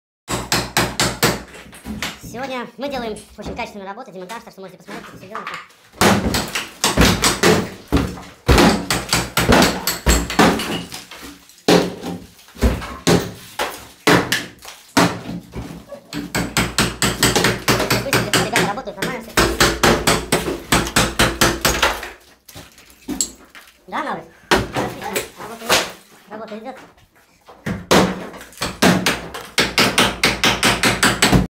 Germanija.Rabota.2016.
Priexali v Berlin i 30 km ot goroda rabotali.Bilo interesno,toljko malo kto prisuwivalsja. A  na samom dele bilo pro4e 4em sej4as. 2021.